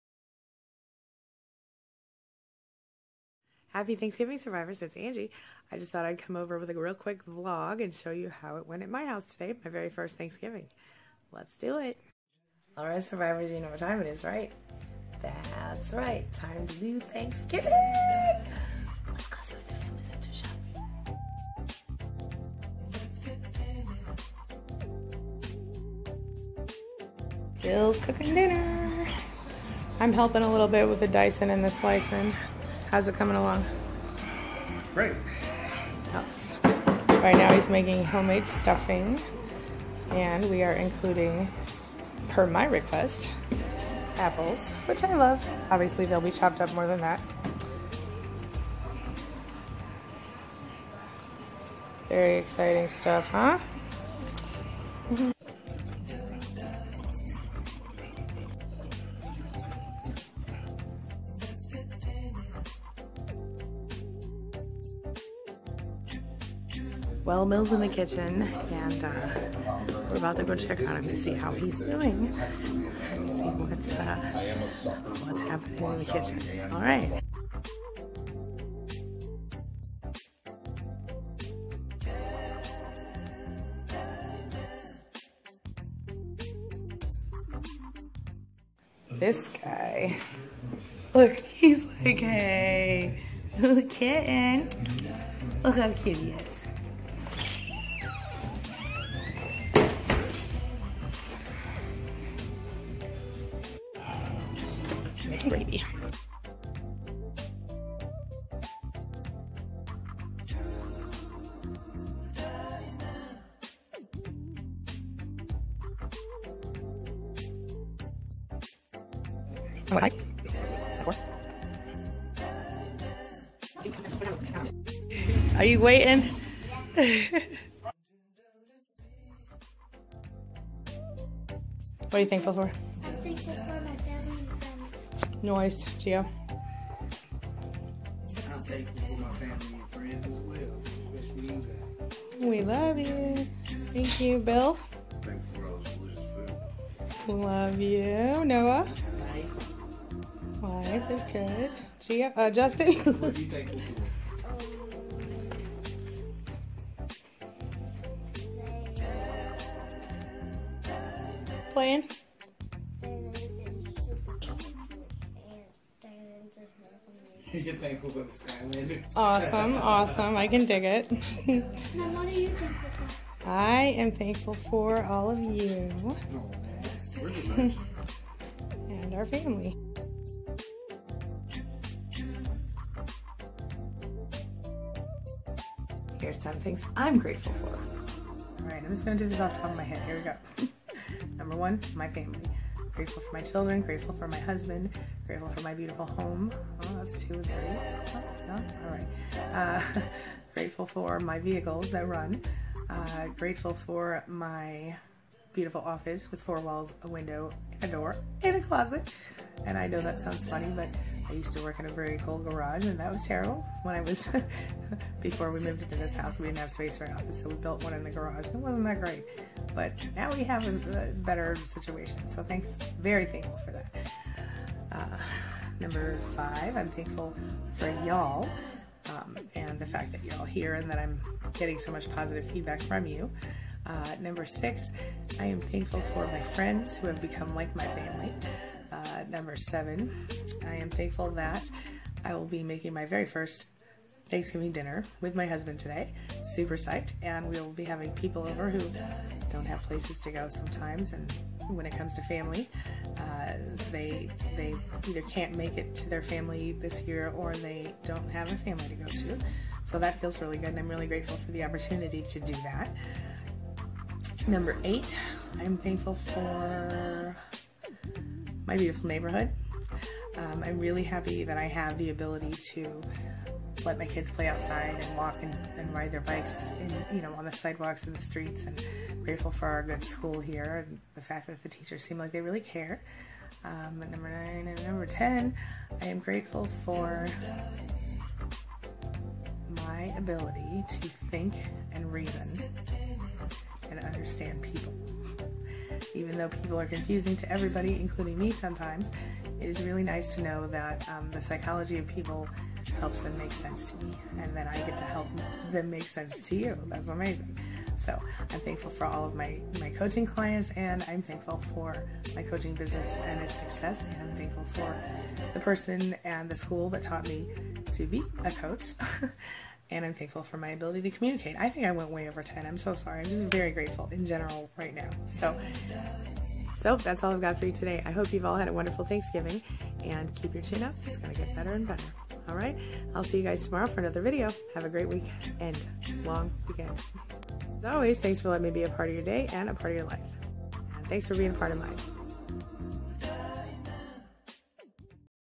Thanksgiving Day at My House: A Family Vlog Special
Thanksgiving Day at our new House: A Vlog Special

Welcome to my first Thanksgiving dinner at our new house! Join me for cooking, dinner and gratitude with my family and family-like friends!